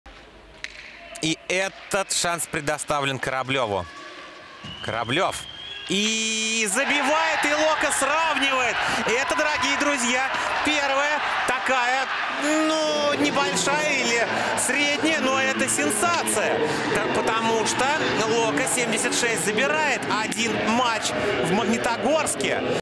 Буллит Кораблёва принес победу «Локо-76». Счет в серии со «Стальными Лисами» сравнялся - 1:1
Ярославский «Локо-76» переиграл «Стальных Лисов» по буллитам во втором матче серии. Команды переезжают в Ярославль. Счет в серии – 1:1. Победный буллит на счету Владимира Кораблёва